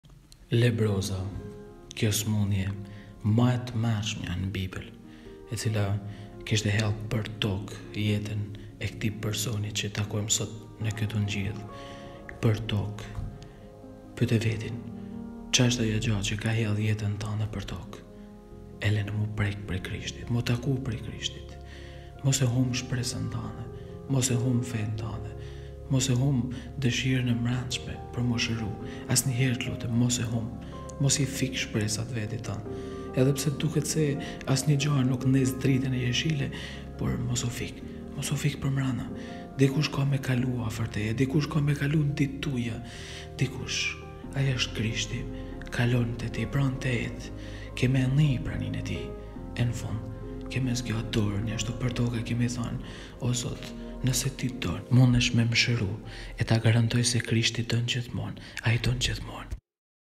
Ungjilli i ditës në 1 minut. Nëse ti do mund të më shërosh!
O Zot, nëse Ti do, mund të më shërosh.
Kjo është lutja e njërit prej atyre që jeta i ka rrëzuar përtokë, i ka lëne pezull e të varur vetëm tek plagët e veta. Personi i gërbulur i Ungjillit, është i tillë, peng në mishin e vet të coptuar, e për më shumë, e më keq, është i braktisur në një gërbulë lebrotike më të madhe, siç është vetmija, indiferenca. Por as në atë gjendje nuk fiket. Ka shpresë. Prandaj të lutem mos u fik përball të keqes sate, edhe pse të gjithë ikin, Zoti nuk largohet, Ai kalon tek rruga jote, vetëm që ti ta shohësh e tia zgjasësh dorën. A mendon se do të ta lërë dorën thatë?!